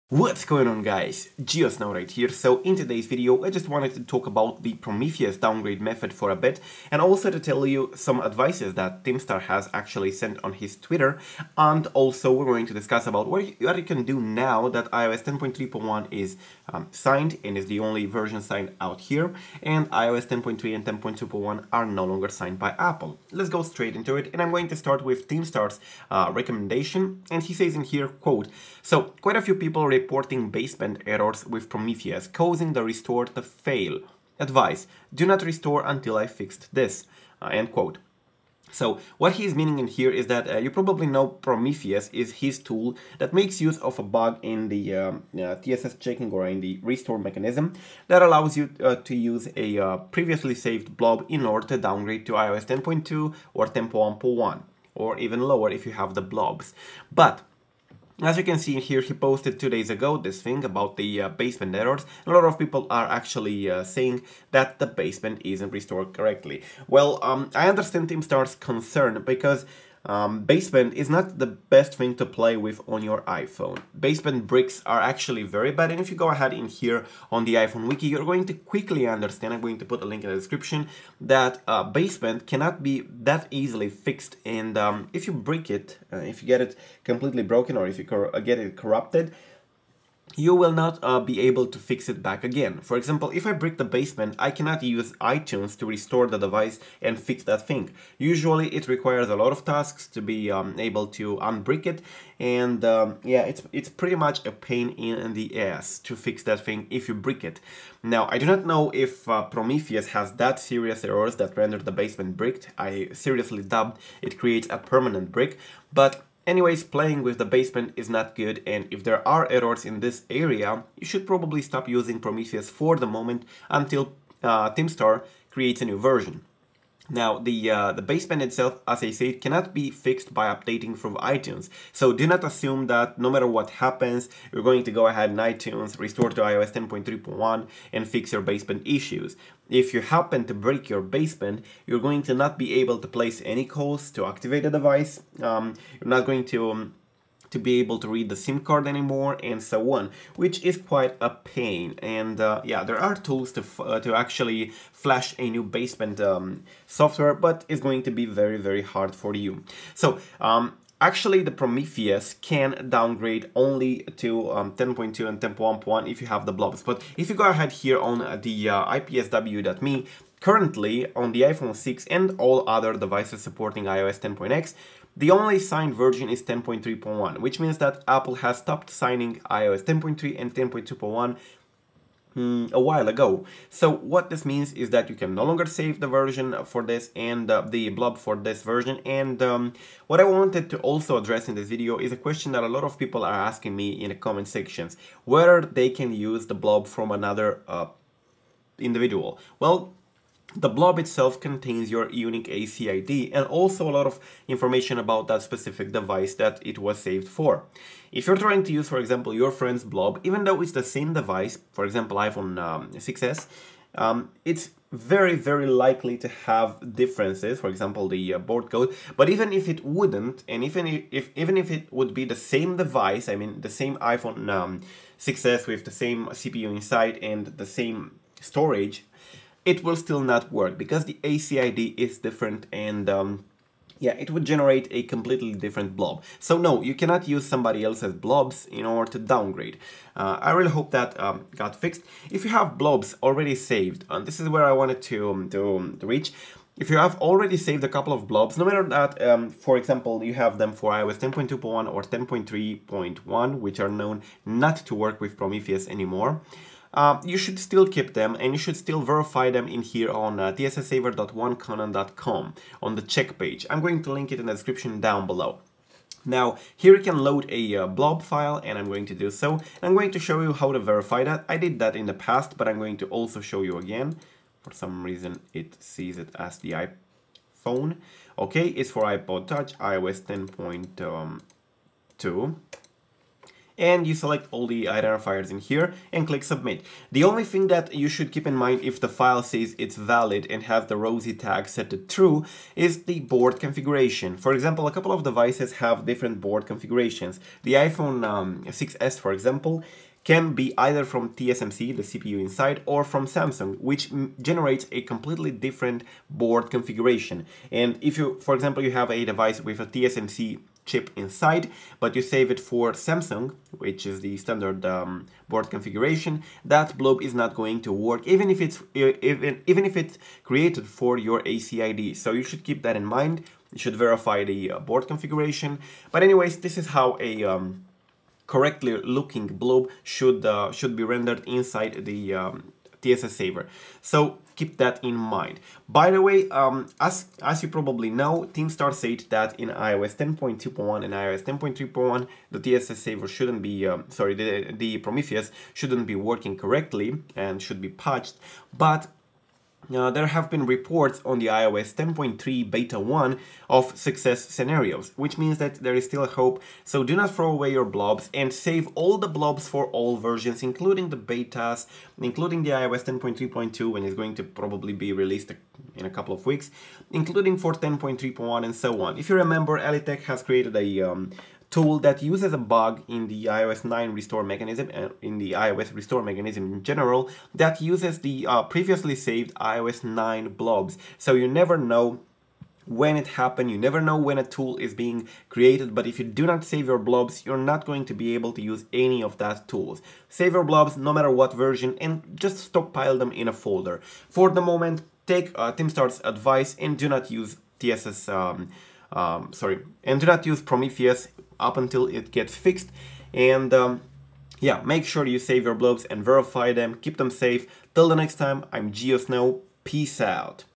iOS 10 Downgrade (Prometheus) | Save Your Blobs! | Baseband Issues
In today's video I have some important things to discuss with you about iOS 10 Downgrades using Prometheus, about the new bugs in the restore process that may result in Baseband issues on the device, and also about the current status of the downgrade. As @tihmstar, the creator of the Prometheus downgrade tool has posted on Twitter 2 days ago, there are some baseband issues currently on the restore with Prometheus, and therefore, downgrade attempts should not be made until @tihmstar fixes the underlying bugs.

Messing around with device's baseband might not be the best thing to do as a baseband brick is very, very hard to fix. Usually if the baseband is being flashed in a wrong / defective manner, and it gets bricked, a normal iTunes restore WILL NOT fix the problem even if you chose to restore to the latest version and even if you erase the device. Normally the baseband has a very different behavior compared to the kernel or other parts that can easily be re-flashed  using iTunes. Baseband downgrades are impossible, and if you brick your baseband, there are slight chances for you to get it back working by using unconventional applications, but even that way, a full recovery might be problematic. Although the restore mechanism used by Prometheus shouldn't be able to provoke such serious baseband issues, @tihmstar still advises us to avoid Prometheus temporarily due to multiple reports of baseband issues he has received.

In the video we also answer one of the most asked question when it comes to downgrades: "Can I use my friend's / someone else's SHSH blobs for downgrade?". I explain in depth why you can't do so and what stops you from crafting a blob. Also, I give you a friendly advice, that is, to save iOS 10.3.1, and iOS 10.3.2 blobs and all the future releases, because bugs like the ReRestore one that allowed iOS 10 to iOS 9 downgrades via SHSH might occur in the future, unfortunately all this bugs are useless without the SHSH files saved. So just because you can't use Prometheus with iOS 10.3.1 SHSH, that doesn't mean a method won't be created in the future.

I am also showing you how to test your SHSH2 blobs to make sure they are correctly saved.